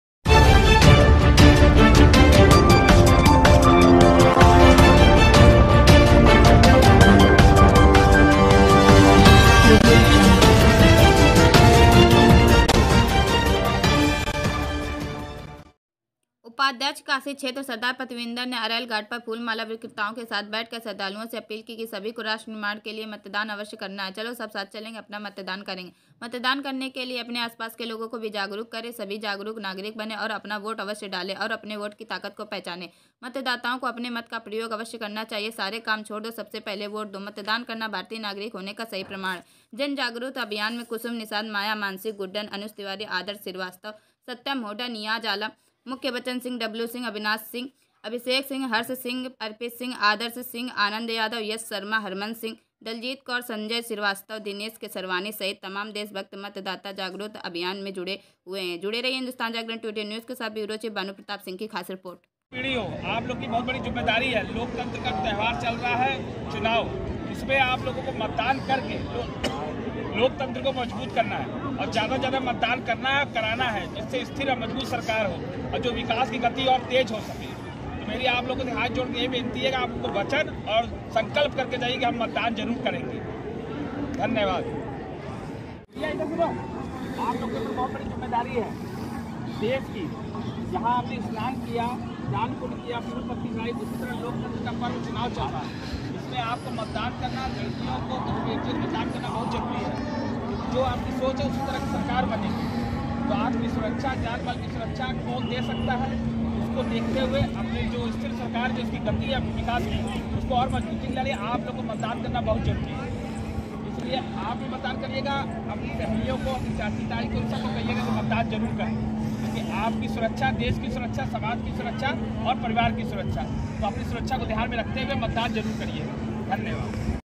उपाध्यक्ष काशी क्षेत्र सरदार पंतविंदर सिंह अरैल घाट पर फूल माला विक्रेताओं के साथ बैठकर श्रद्धालुओं
उपाध्यक्ष काशी क्षेत्र सरदार पंतविंदर सिंह अरैल घाट पर फूल माला विक्रेताओं के साथ बैठकर श्रद्धालुओं से अपील की  सभी को राष्ट्र निर्माण के लिए मतदान  अवश्य करना है चलो सब साथ चलेंगे अपना मतदान करेंगे। मतदान करने के लिए अपने आसपास के लोगों को जागरूक करें सभी जागरूक नागरिक बने और अपना वोट अवश्य डालें अपने वोट की ताकत को पहचाने, मतदाताओं को अपने मत का प्रयोग अवश्य करना चाहिए। सारे काम छोड़ दो सबसे पहले वोट दो, मतदान करना भारतीय नागरिक होने का सही प्रमाण है।